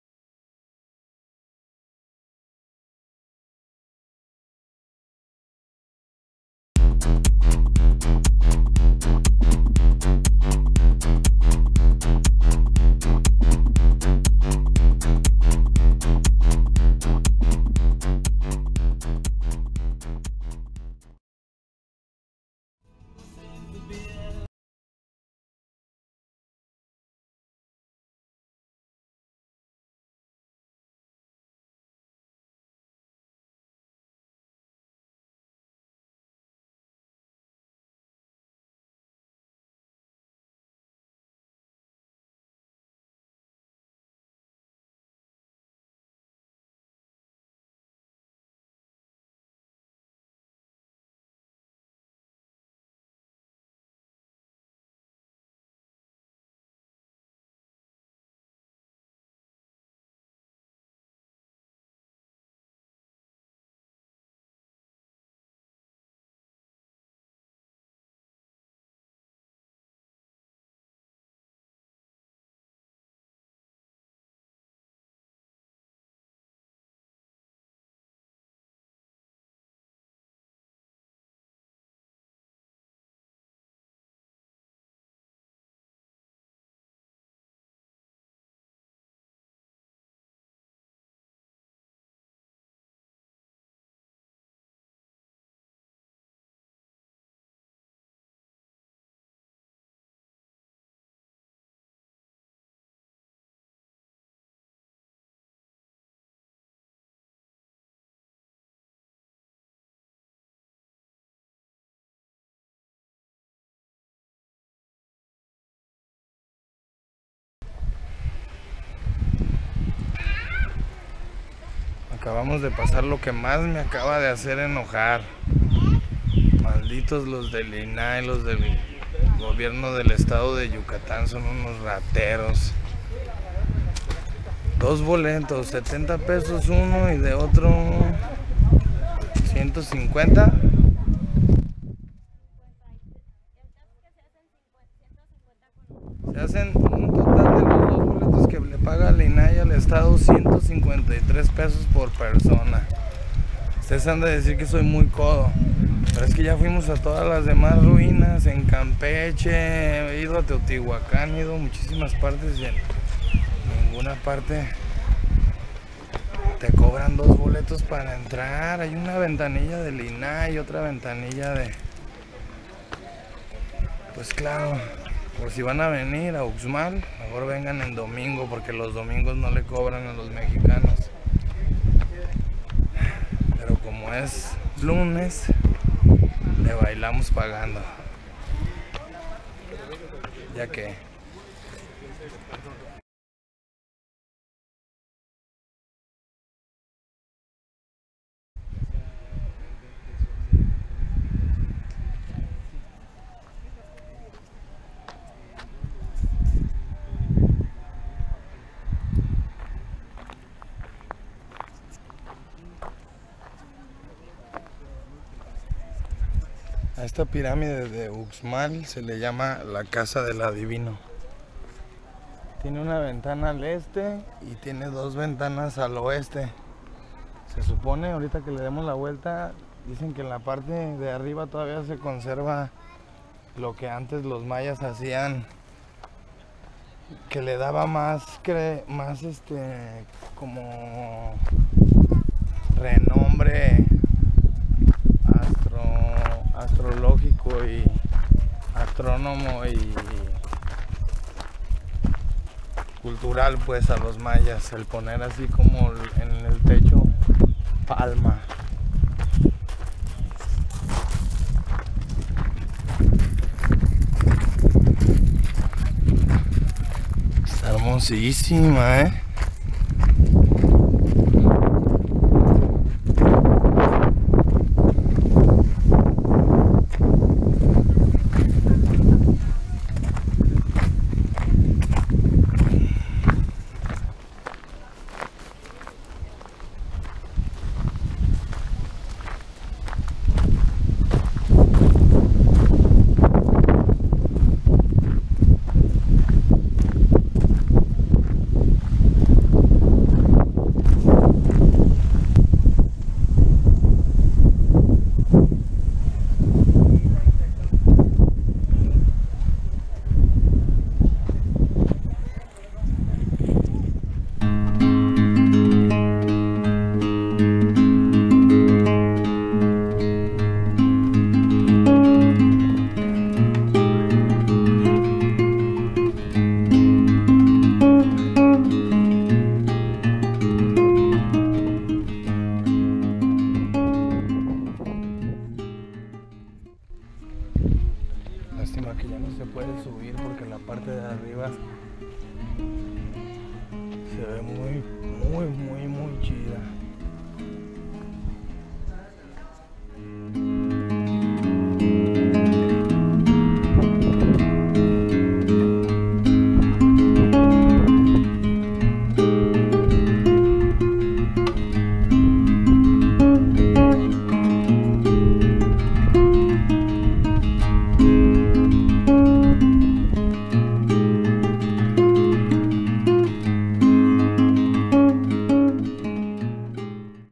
UXMAL más bonita que CHICHEN ITZA (Recorrido Ruta Puuc)
Uxmal es una Antigua Ciudad Maya en el Estado de Yucatan en México.
Generalmente poco Visitada ya que la gran representación maya en pirámides es CHICHENITZA.
Mas sin embargo sin duda un lugar para visitar.
una gran pirámide de nombre:
LA PIRÁMIDE DEL ADIVINO.